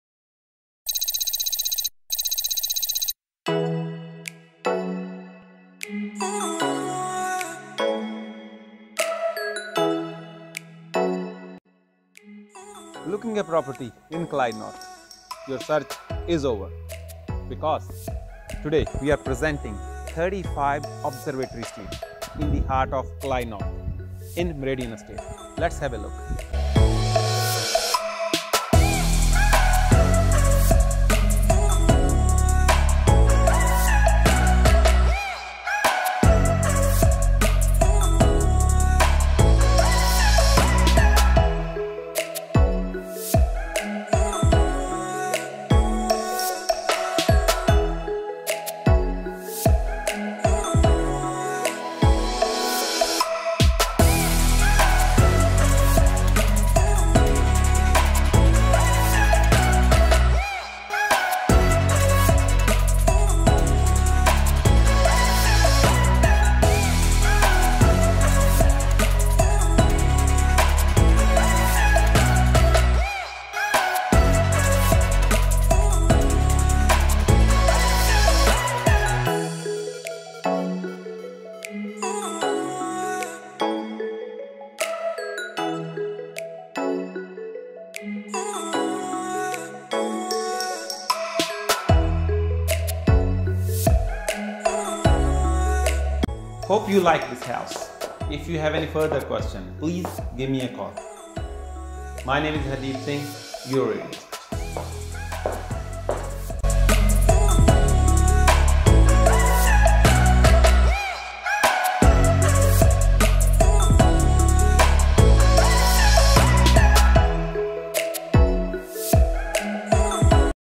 35 Observatory Street Clyde North
Clyde North is already a booming market for investors and for new families to reside in.
Main Features of the Property:
- 448sqm(approx.)
- 4 Bedrooms
- Master with Full Ensuite
- Walk in Robe
- Built in Robes
- Study
- 2 Living Areas plus Dining
- Stone Bench Tops
- Tiled Splash Back
- Pantry
- Ample Storage
- Laundry
- Linen
- Alfresco
- Double Car Remote Garage
- Internal Access

Heating: Yes
Cooling: Yes
Downlights: Yes
Dishwasher: Yes

This property stands on an ideal location close to all amenities:
- Eden Rise Shopping & Casey Central
- The Avenue Village Shopping Centre
- Selandra Rise Shopping Centre
- Bunnings
- Food Courts
- Public Transport
- Train Station
- Local Schools
- Childcare Centres